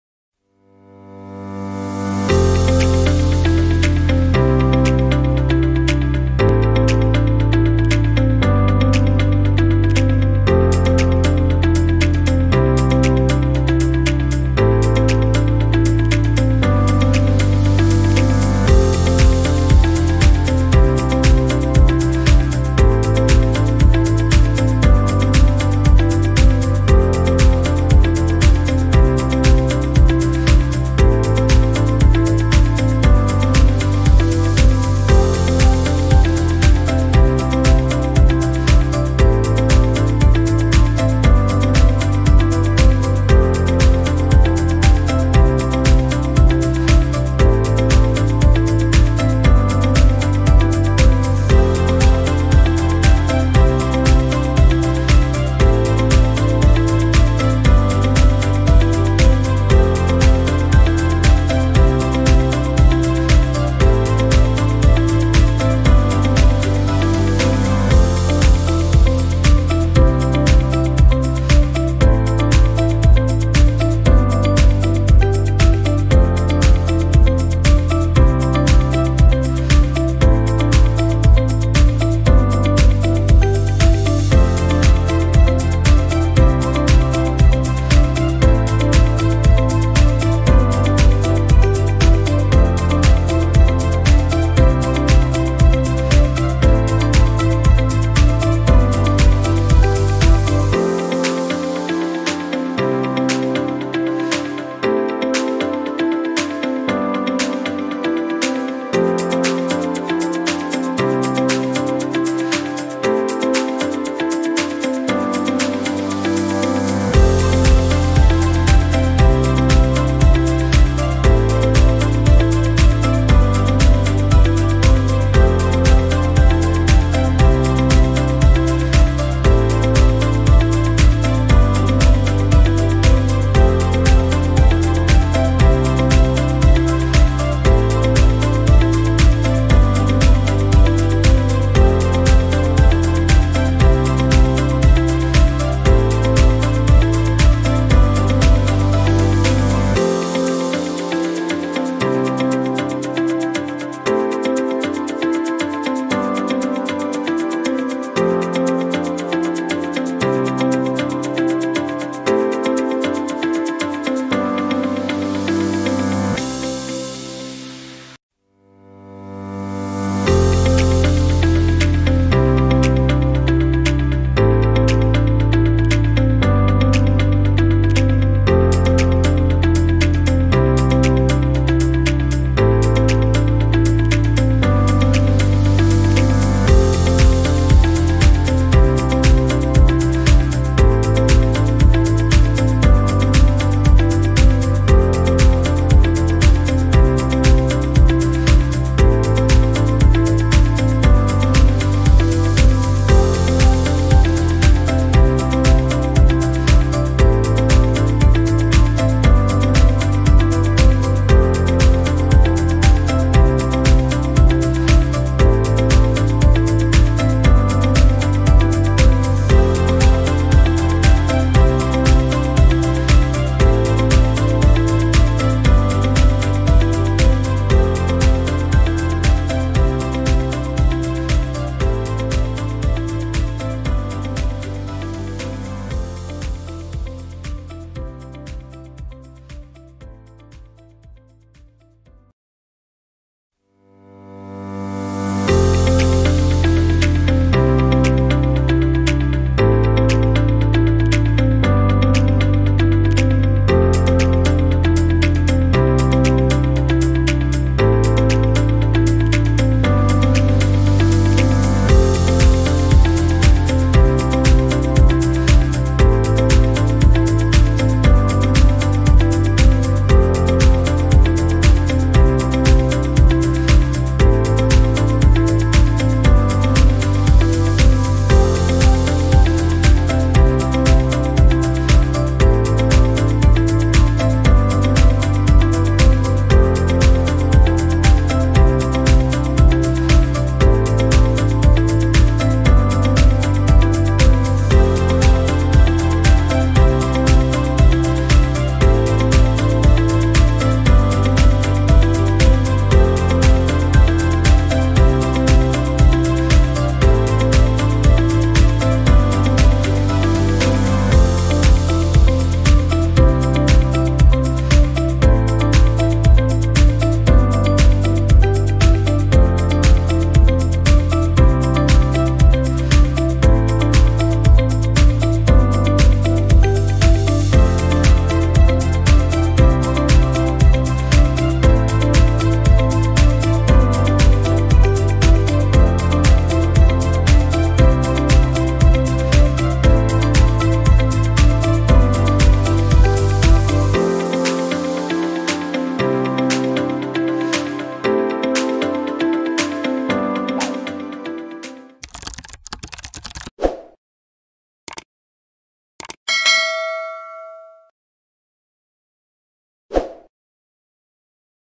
Installing 4MLinux 38.0 in VirtualBox
A tutorial video showing the installation of 4MLinux 38.0 STABLE inside VirtualBox.